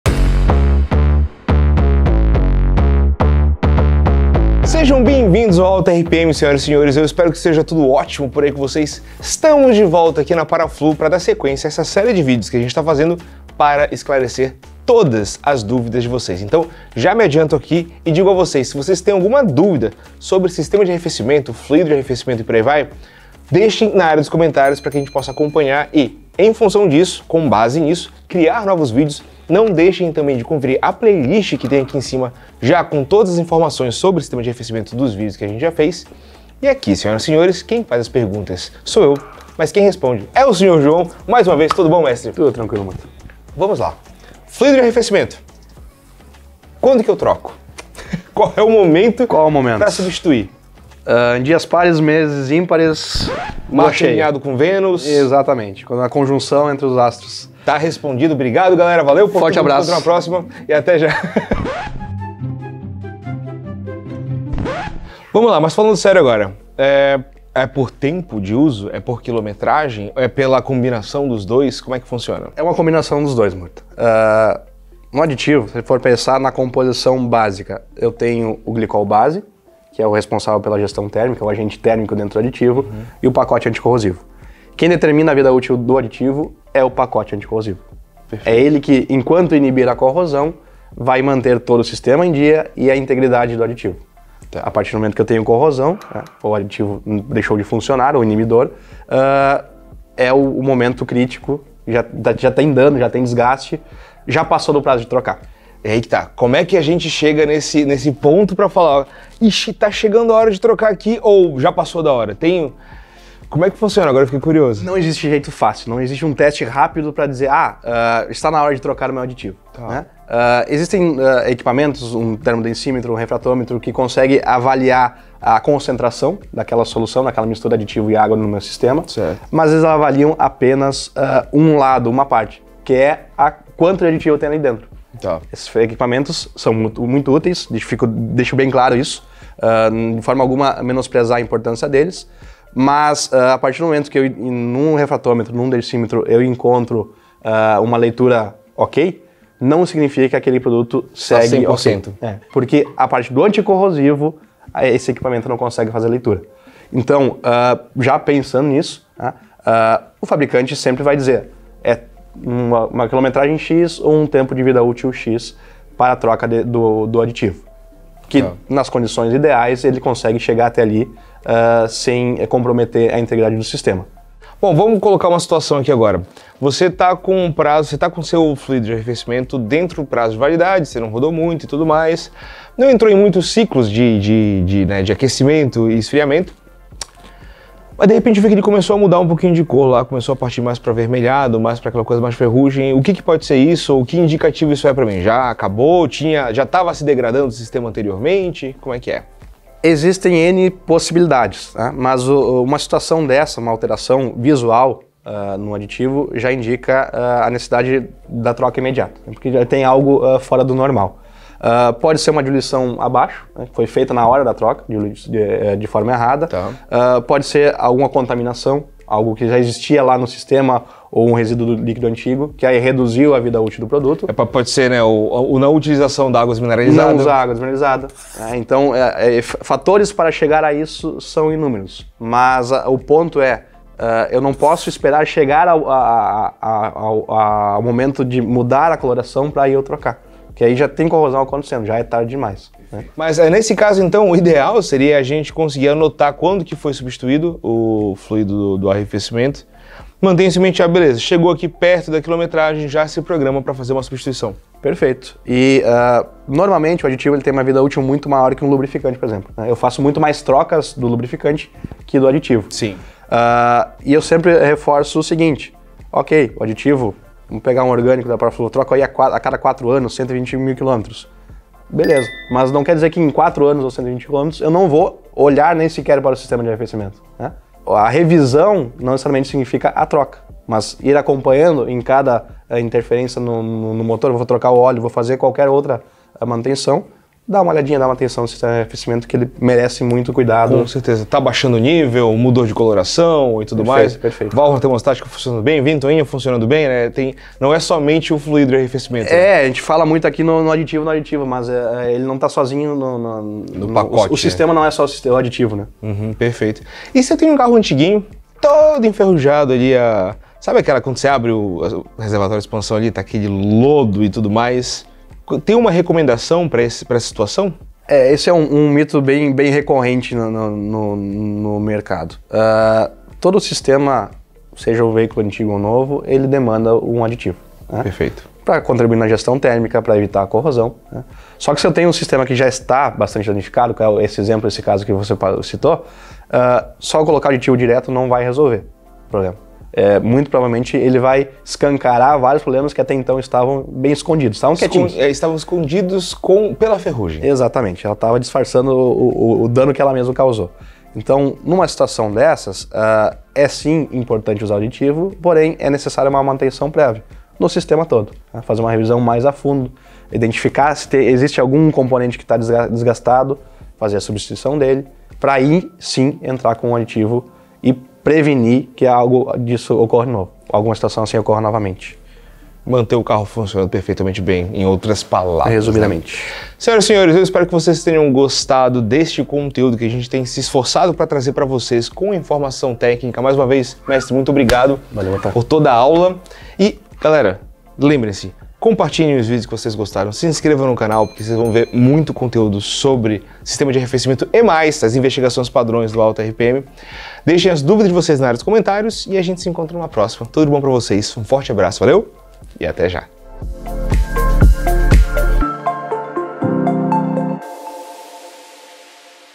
FLUÍDO DO RADIADOR: QUANDO TROCAR? ALTA RPM
Entre verdades e lendas urbanas... Quando é o momento adequado para trocar o fluído de arrefecimento do motor de um carro? Para responder a questão, contamos com as orientações da Paraflu!

Clique no play e  confira!

Siga a Paraflu no instagram: @paraflubr
Siga a Paraflu no YouTube:  @Paraflu

Siga a Pneu Free no YouTube:  @pneufree.com